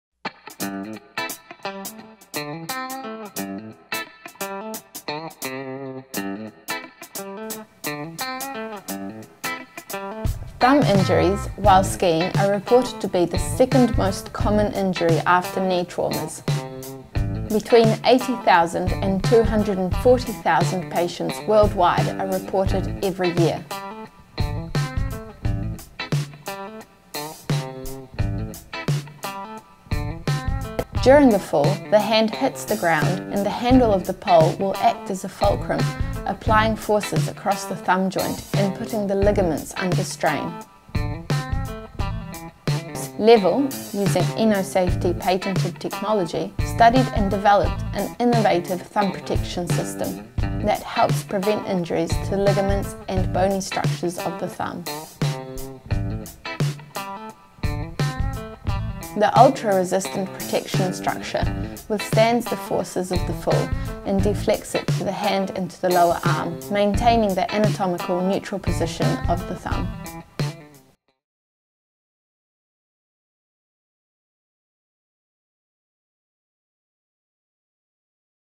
Level Thumb Protection System
Hand injuries in general tend to be disregarded by both patients and doctors, until it happens to oneself. Never was this more the case than in thumb injuries, as the importance of the thumb is usually under appreciated, until injured.

In skiing they are reported to be the second most frequent injury after the knee. In detail thumb injuries are reported to make up between 5% - 20% of all injuries in Alpine skiing and are sustained by approximately 80,000-240,000 patients per year world wide.

THE GOAL OF THE NEW LEVEL THUMB PROTECTOR IS TO HELP PREVENT INJURY TO THE UCL AND BONE STRUCTURES OF THE THUMB.